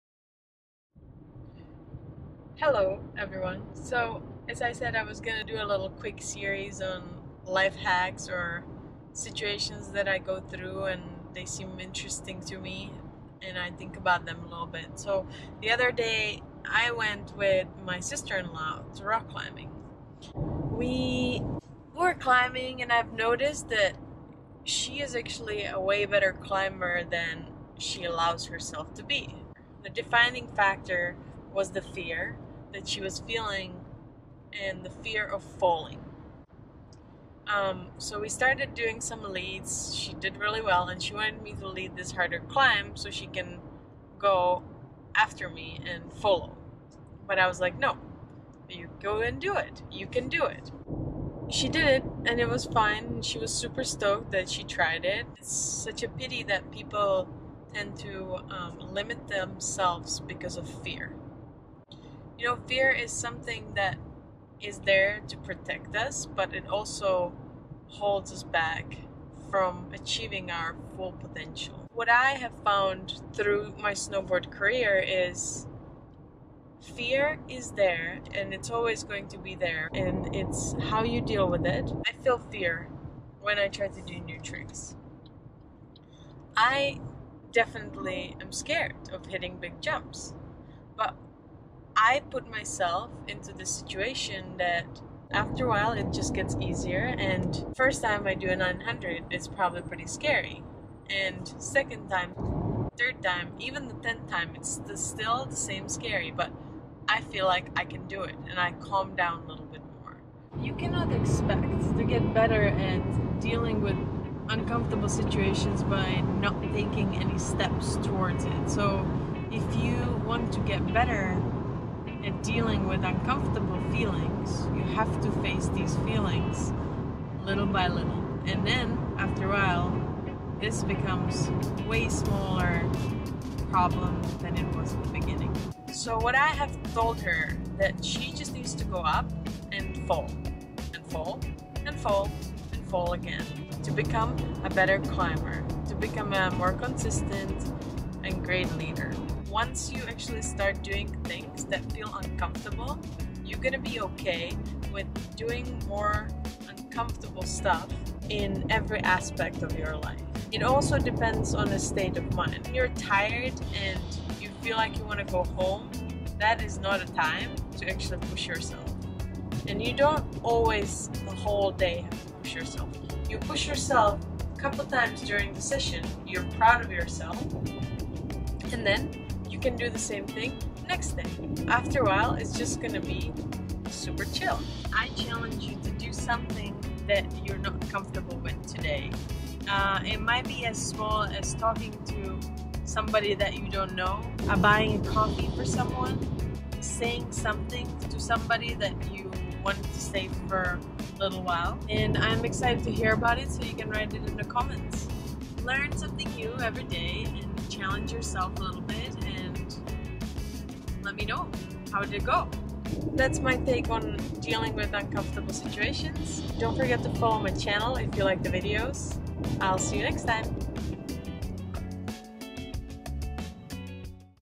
Shark Tales vlog 48 Comfortable with uncomfortable
Another part of my self improvement series.
This one is about feeling comfortable wit the uncomfortable.
We all have times when we are scared of something. It could be in athletics or real life.
Here is how I challenge myself. Its never easy to push your boundaries. I do certain steps that make it easier.
And you can watch it here.

Edited by: Sarka Pancochova